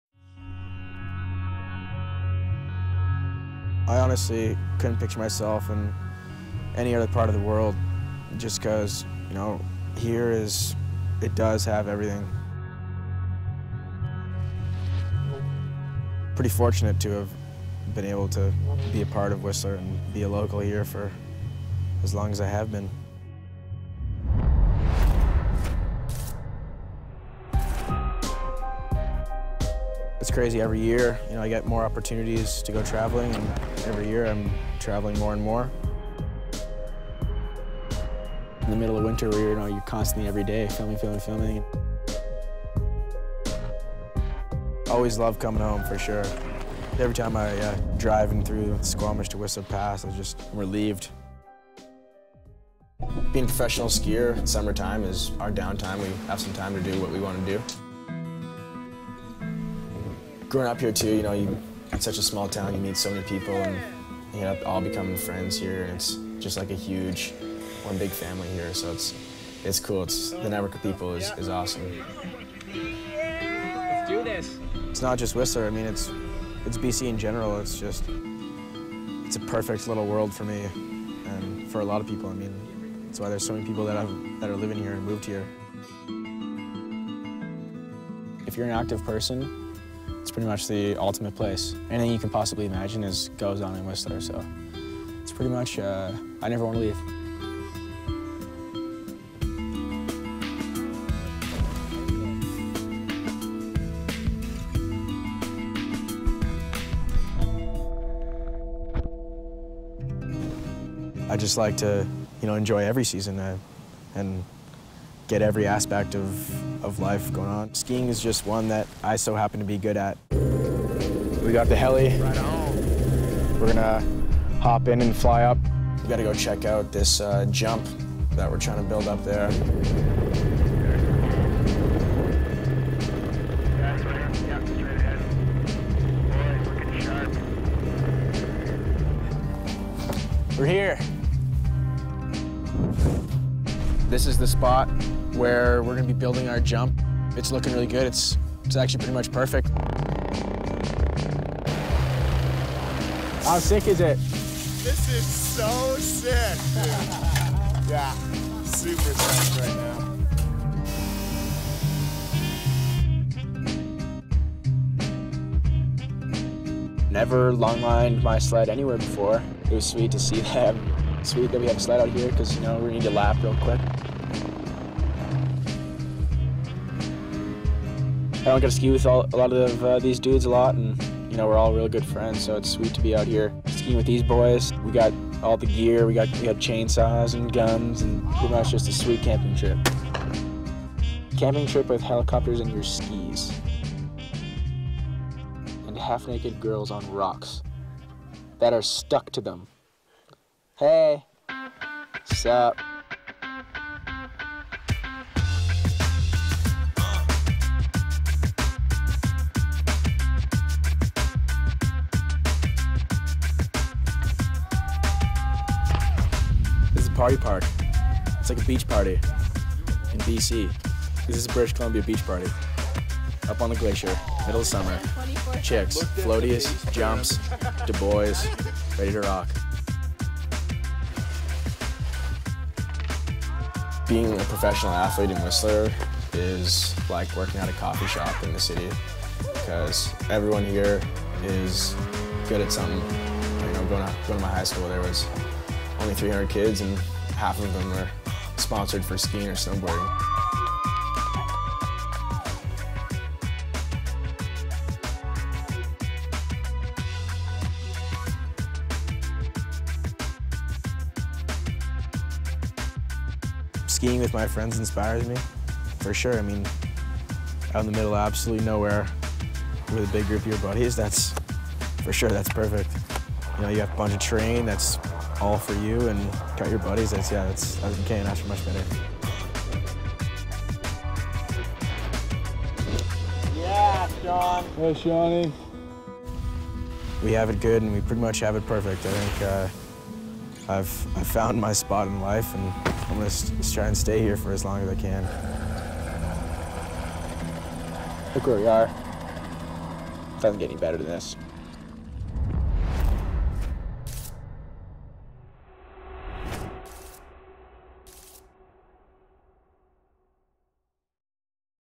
Helicopters, Skis, and Half-Naked Girls - Red Bull Playgrounds 2012
Meet Canadian freeskier and Red Bull athlete Sean Pettit, a 20-year-old prodigy who was fortunate enough to grow up in the beautiful Whistler, BC. We take an inside look at Sean's day to day life, and see why Whistler is the ultimate location for any active individual.